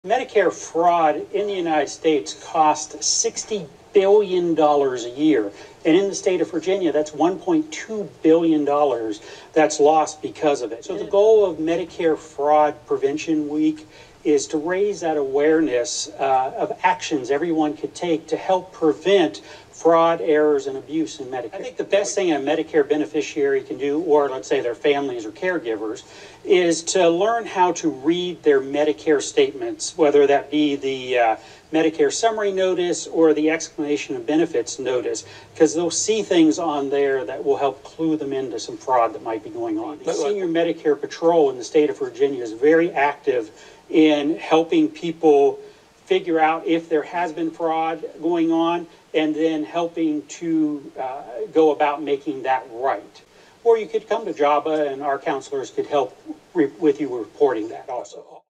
JABA raising awareness about Medicare fraud
It's Medicare Fraud Prevention Week MFPW and Randy Rodgers, who manages our Insurance Counseling Program, spoke with Steve Rappaport at 29News about the problem and what people can do about it.

"Medicare Fraud in the United States costs 60 billion dollars a year, and in the state of Virginia that 1.2 billion dollars that's lost." said Rodgers, "and it affects the individual who is on Medicare in terms of stress in their life, the time they have to use in combatting this, even their health."

Rodgers said it's important to keep track of the health services you receive and read your Medicare statements carefully, looking out for mistakes and charges you don't recognize.